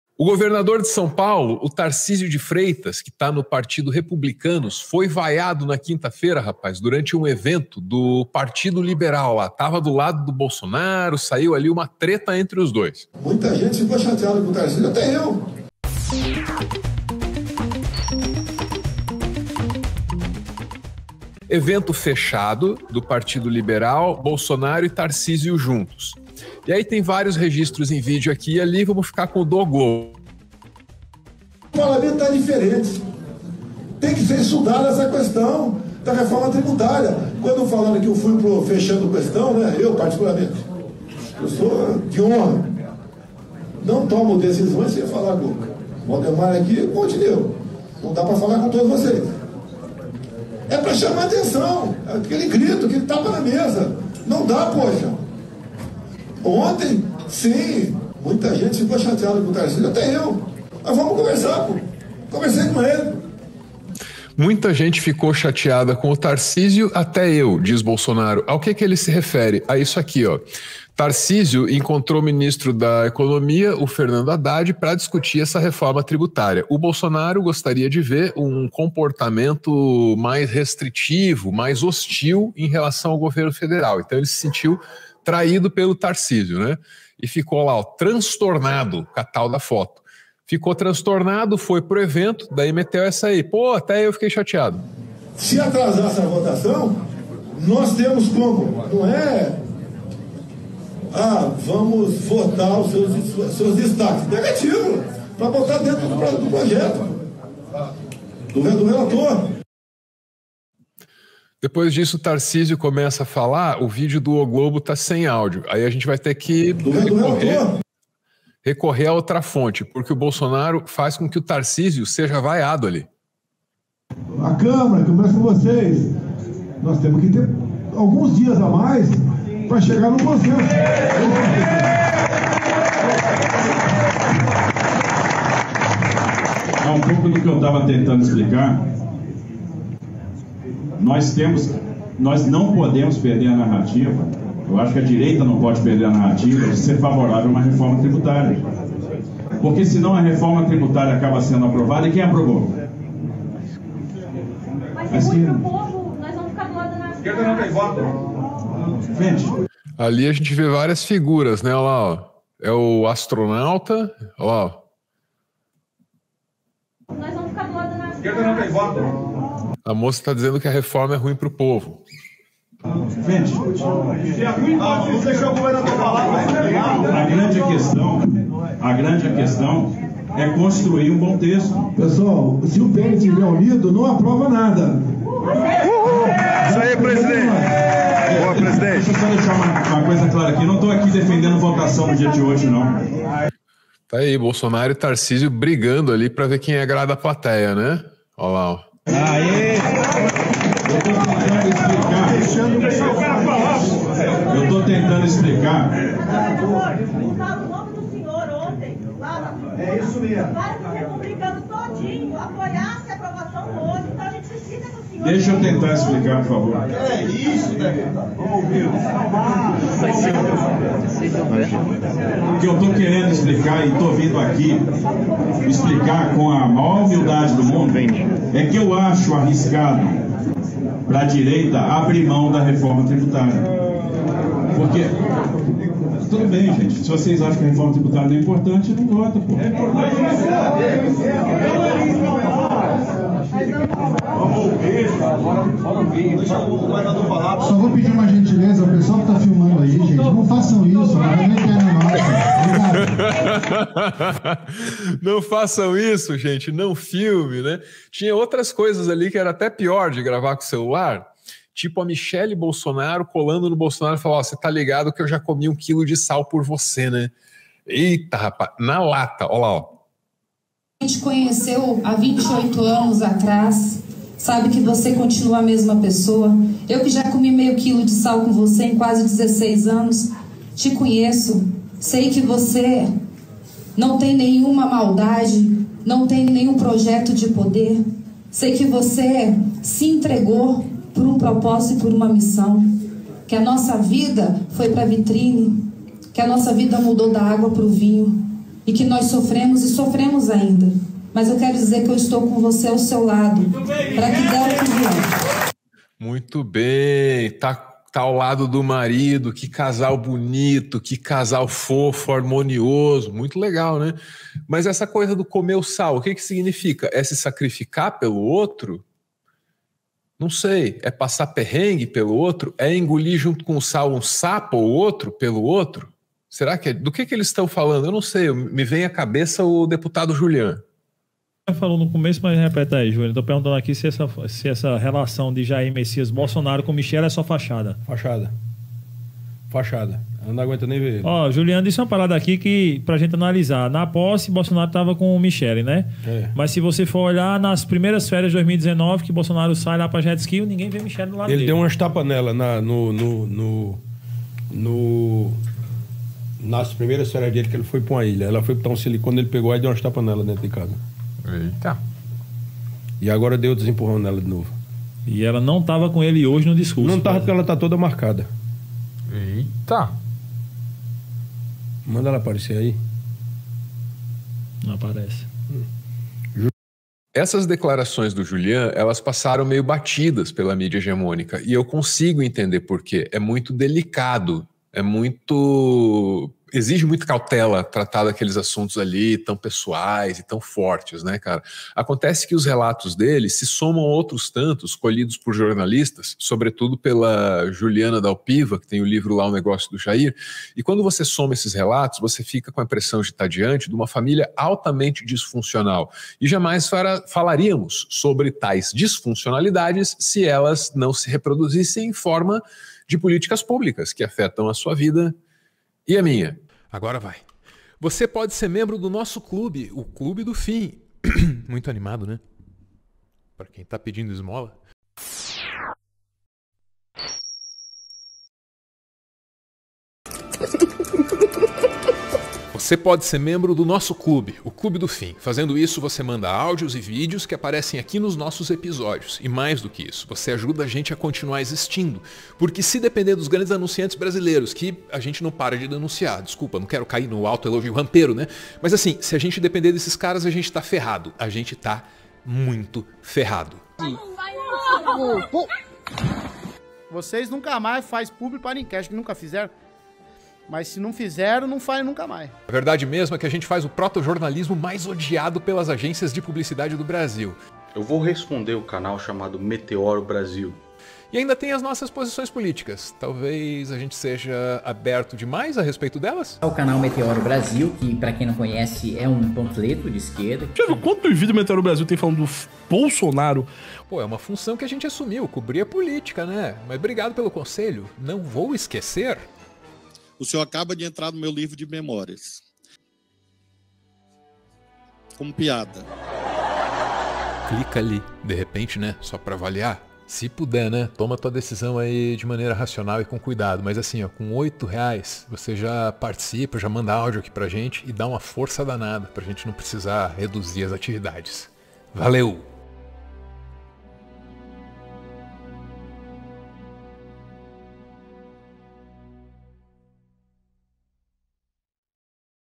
VAZOU! BOLSONARO BRIGA COM TARCÍSIO EM EVENTO FECHADO
O governador de São Paulo, Tarcísio de Freitas (Republicanos), foi vaiado nesta 5ª feira durante evento do PL depois de se manifestar favorável à reforma tributária.

Seja membro do nosso clube de canais, você nos ajuda a continuar existindo e manda mensagens que podem aparecer nos nossos vídeos: